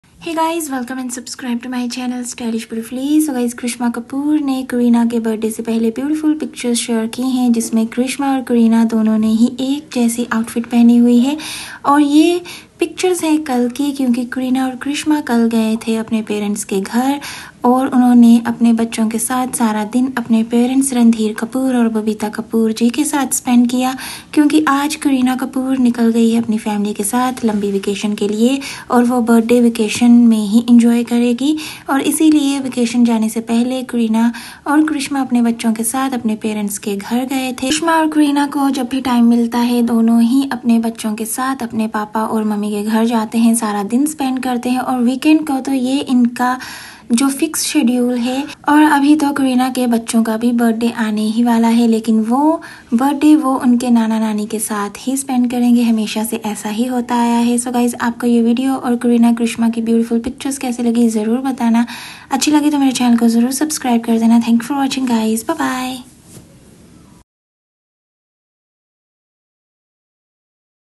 Karisma Kapoor kareena kapoor wearing same outfit at papa Randhir Kapoor house and having fun
Kareena Karisma Kapoor papa Randhir Kapoor is spotted and both sisters worried for papa

Kareena Kapoor Karisma Kapoor at parents house to meet Randhir Kapoor and Babita Kapoor

Kareena Kapoor with Taimur and jeh Ali khan spending time and doing shooting after vacation